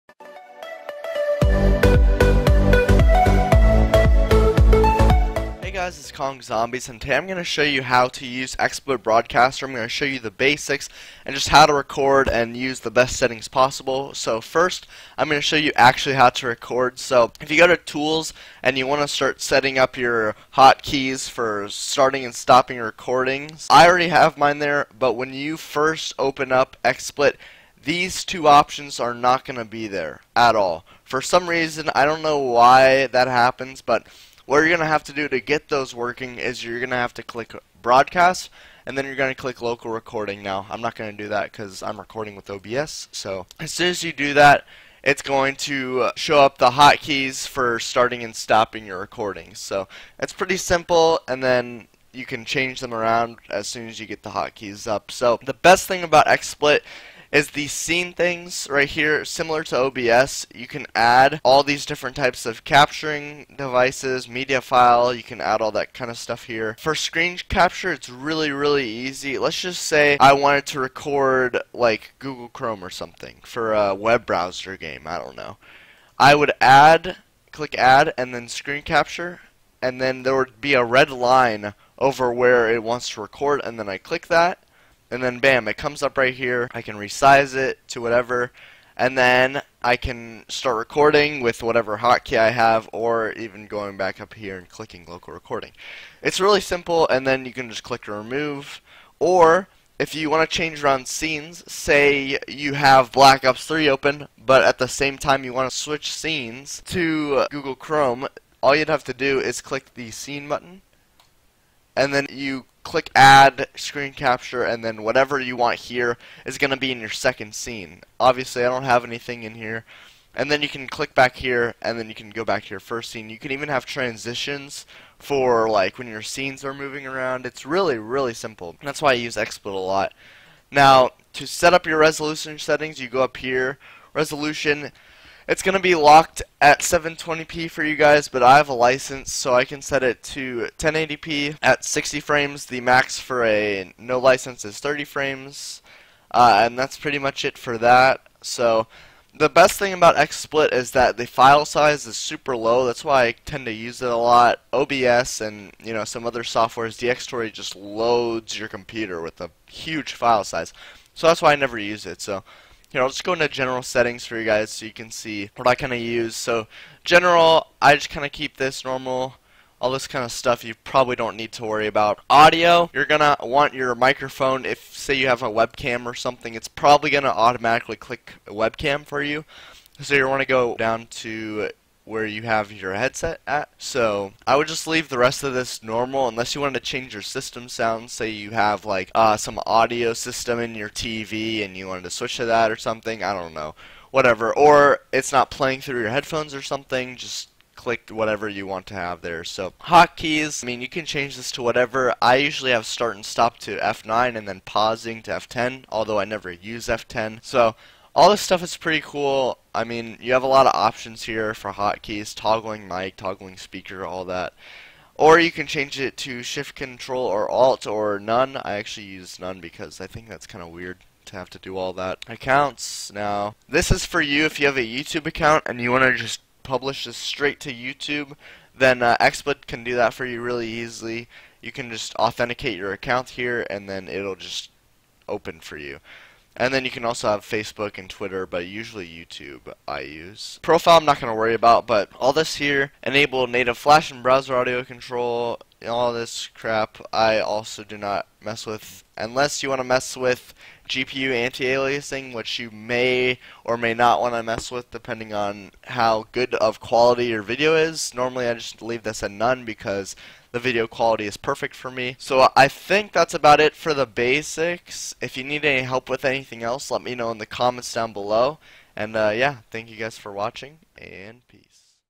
How to Use Xsplit Broadcaster - The Basics 2017 (PC)
In this video I show you guys how to use Xsplit Broadcaster and show the basics of it. When I first got Xsplit Broadcaster, I was confused at first because there were many different options that I didn't quite understand. Now, after using this software for a long time now, I know how to use it properly and effectively, and am here to show you how to use it as well. Hopefully this video helps!